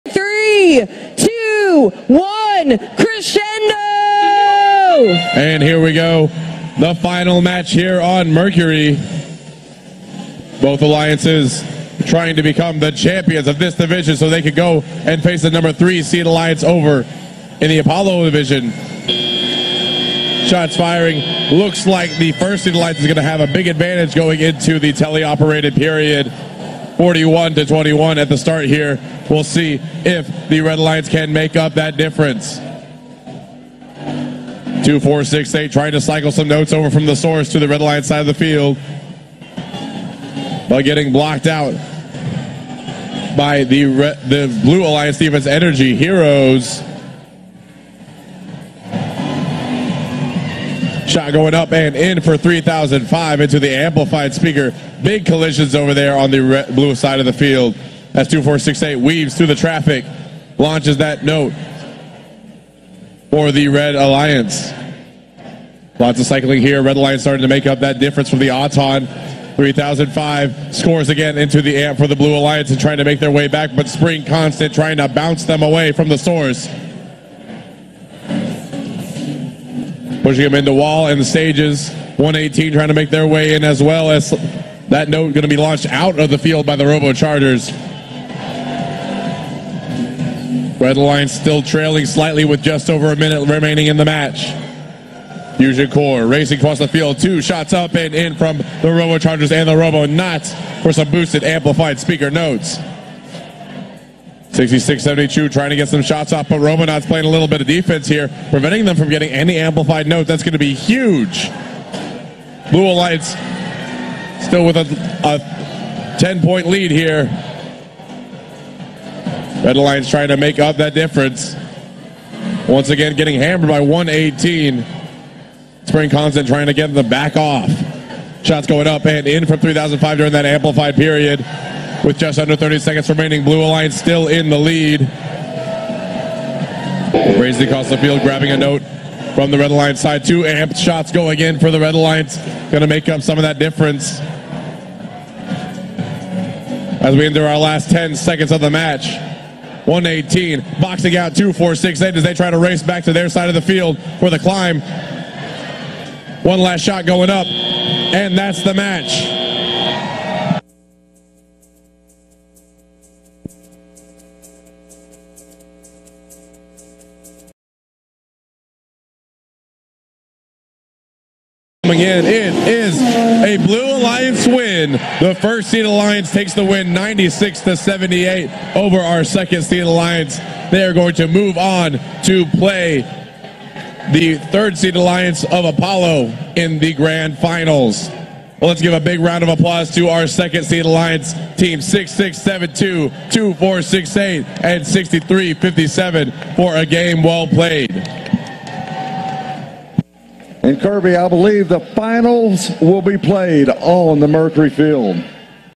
Mercury 2024 Final Tiebreaker: 118, 3005, 5892 vs 2468, 6672, 6357
FiT Texas Championship Event 2024 - Mercury Division - Final Tiebreaker Match

FIRST Robotics Competition - FRC
Date: 4/6/2024

Red Teams: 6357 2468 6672